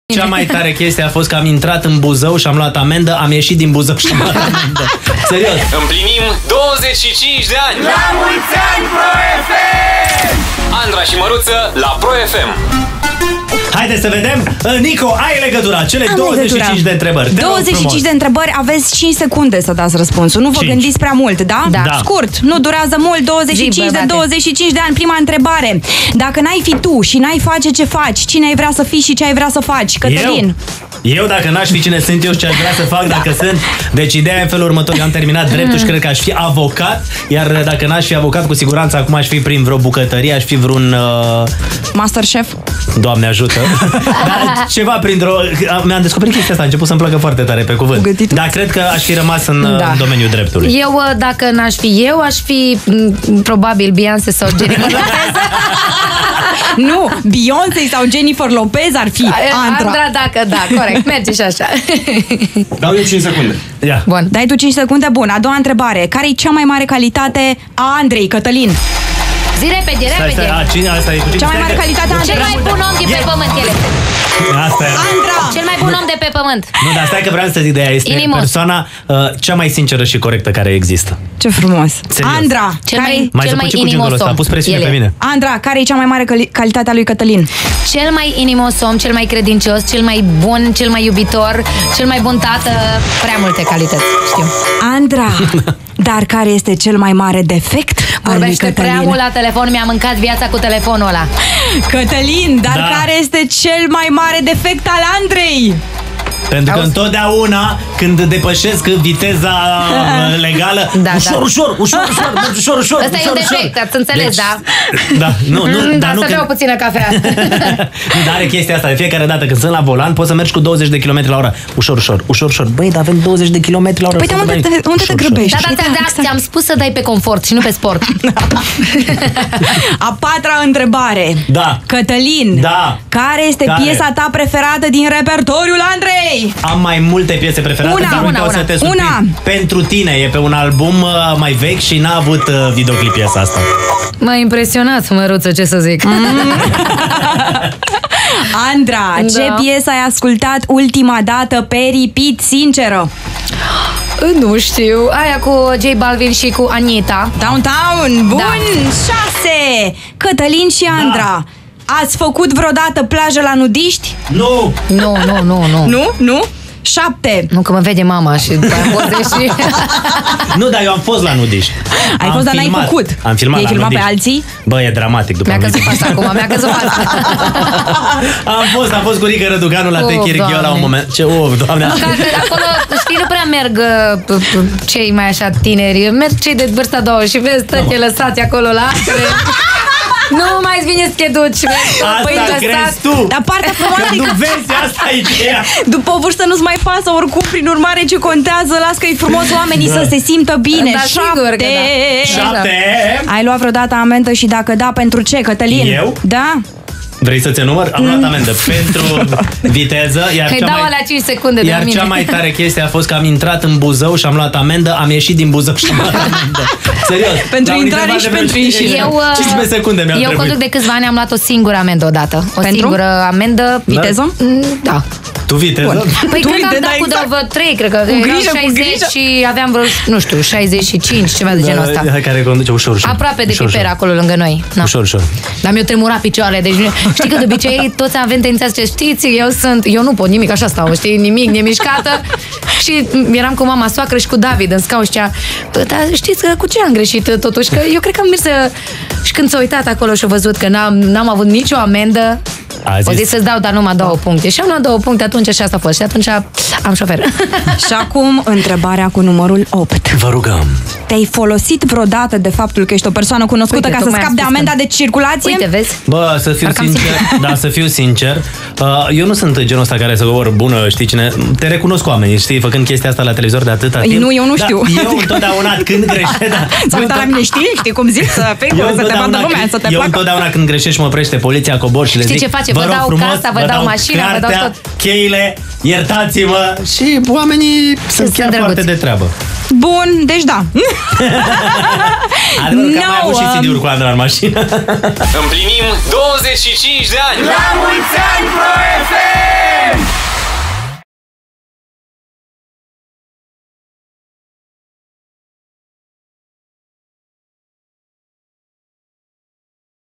Andra si Maruta - Testul cu 25 de intrebari Partea 1
Andra si Catalin Maruta, în direct cu Nico Creciunesc, live la ProFM. La 25 de ani de ProFM ei au facut emisiune la radio, intre 10:00 si 13:00